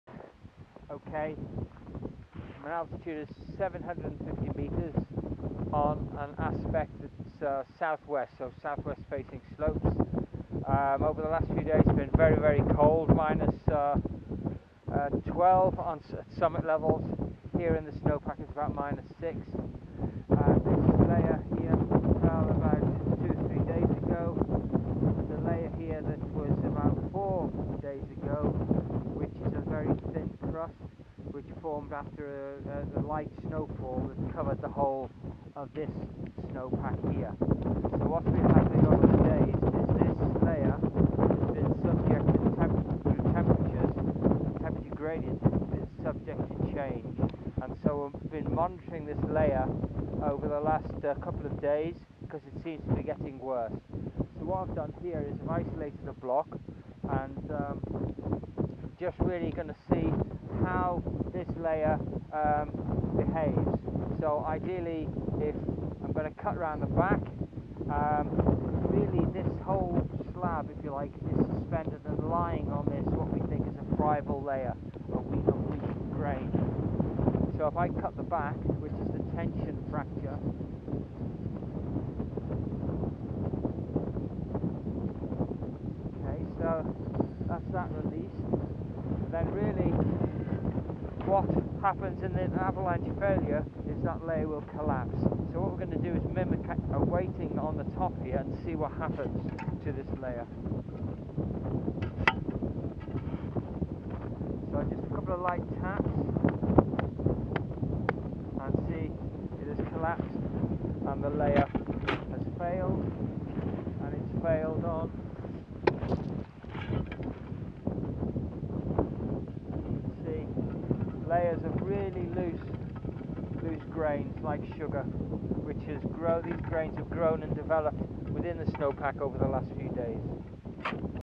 Northern Cairngorms - snowpack observation 31.1.21 , SW aspect 750m alt.
Today went to lower altitude to check if weakness that is fairly widespread above 850m in North Cairngorms is present - video illustrates that it is at lower altitudes especially where windslab has been deposited over a thin crust - steep slopes 35 deg or so and convex in shape would be most likely as a potential release place.